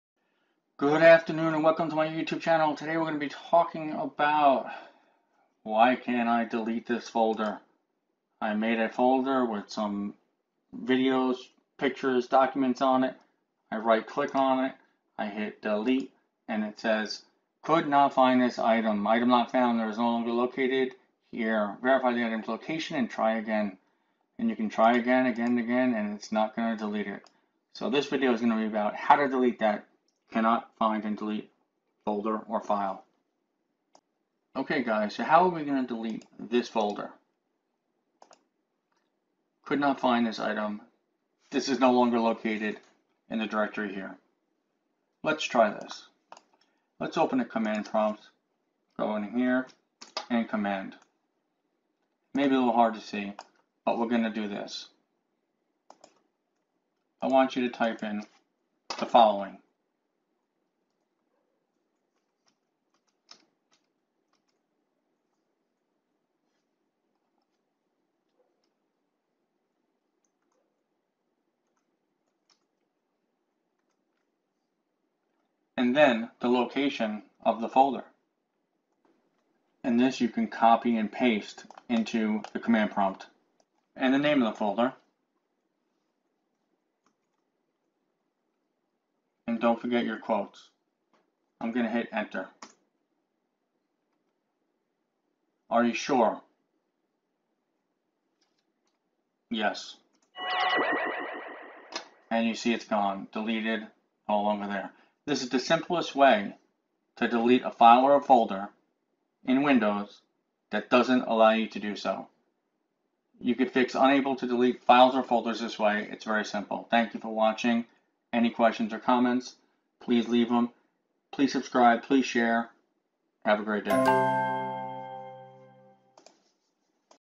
How to Fix Item Not Found Could not find this item, this is no longer located in
This error message is annoying and difficult for some to fix, but if you follow my short video you will fix it quickly.